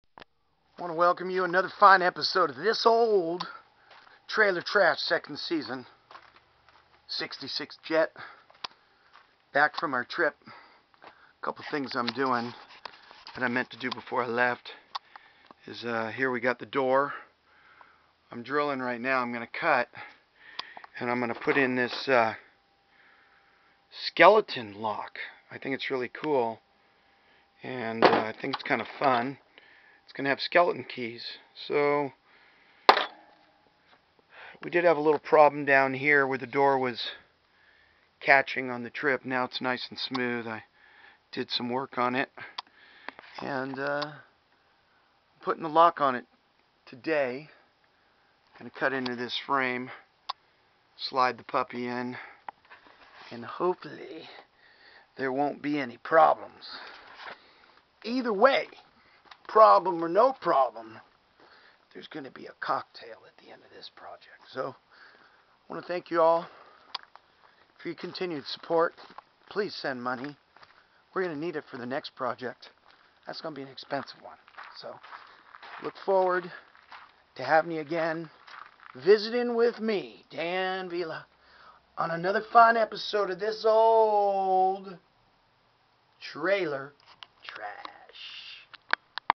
This Old Trailer Trash, 2nd Season, Skeleton Key Lock Install
Now that I'm back there are a few details to finish on the Trailer. A door lock would be nice to have. A painted stripe would also finish it off.